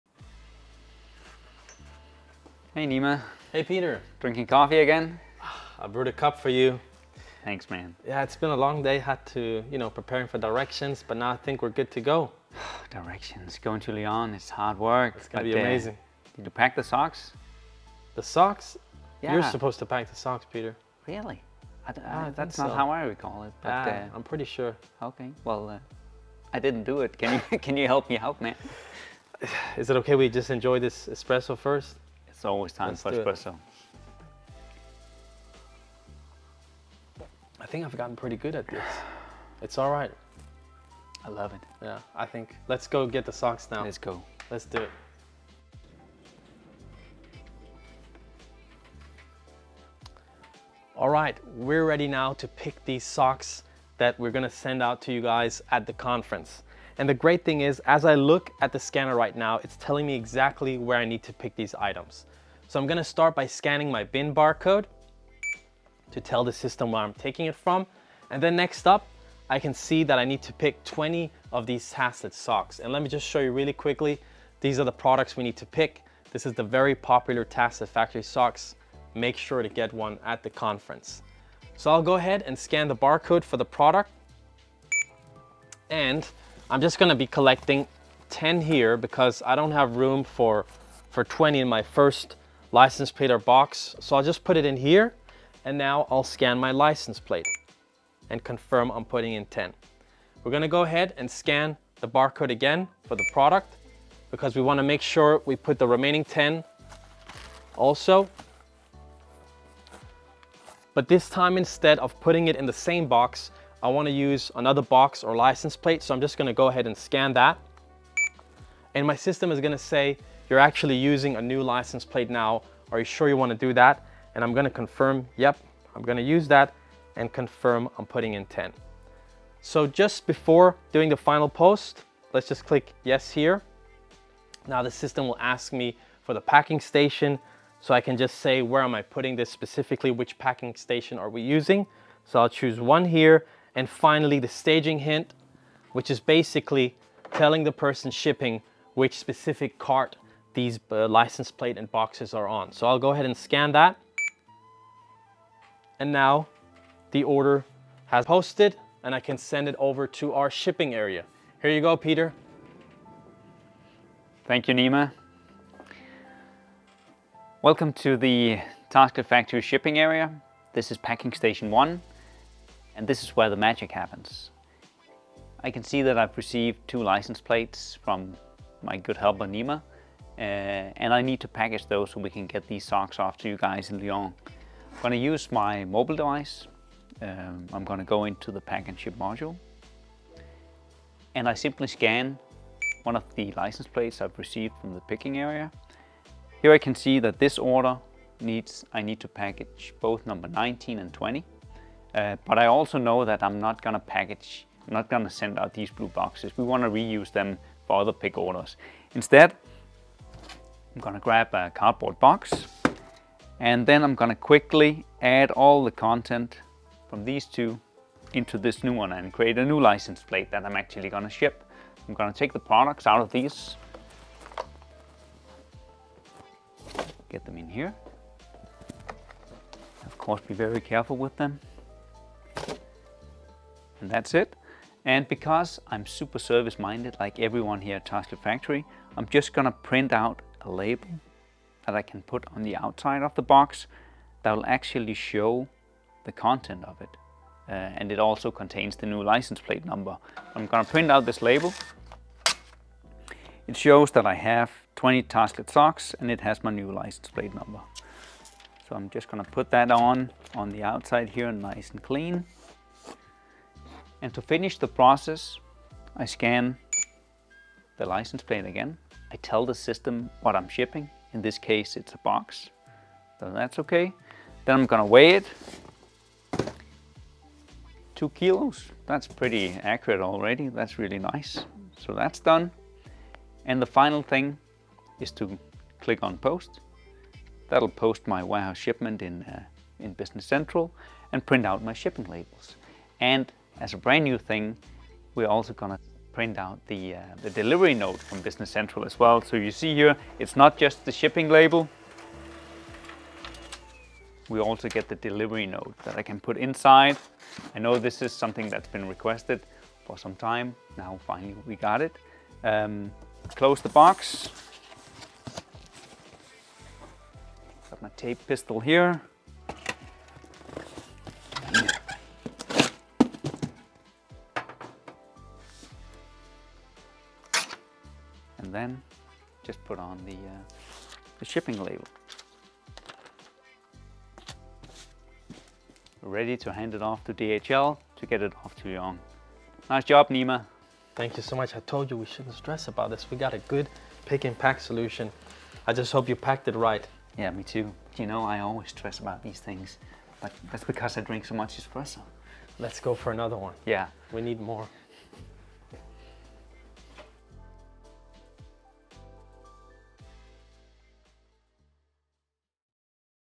Pick, Pack & Ship with Tasklet Mobile WMS
Watch Nima Hamidi and Peter List demonstrate how to pick, pack & ship your order with Tasklet Mobile WMS. This video was shown during the Tasklet presentation at Directions EMEA 2023. Enjoy!